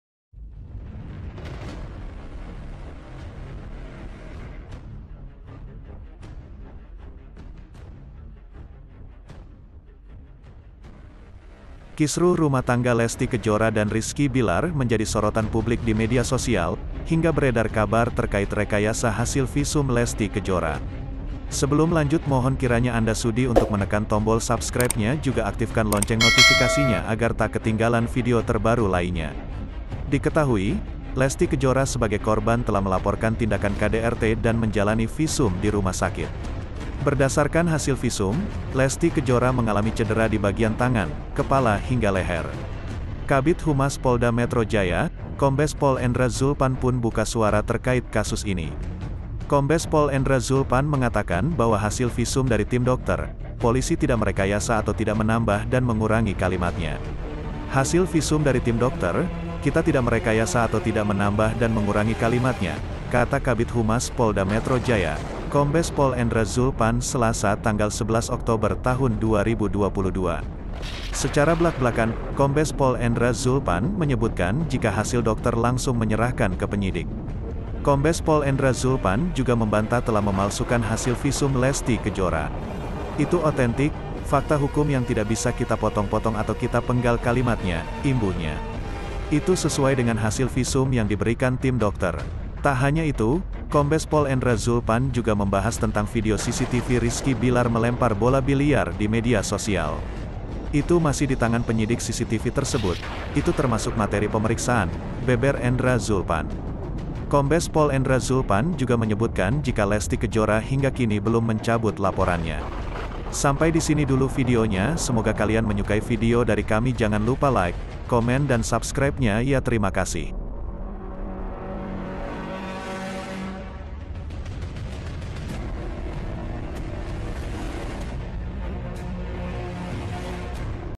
Tak Lagi Diam !! Dituding Palsukan Hasil Visum Lesty Polisi Beri Jawaban Menohok Ke Pengacara Billar
lesty kejora
rizky billar
berita terkini